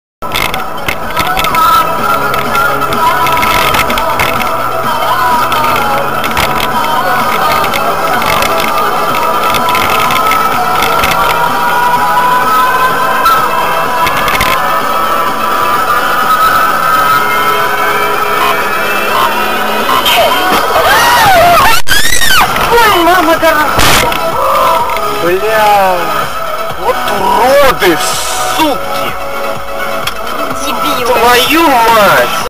Motoqueiro Russo
Na Russia o motoqueiro atropela o carro...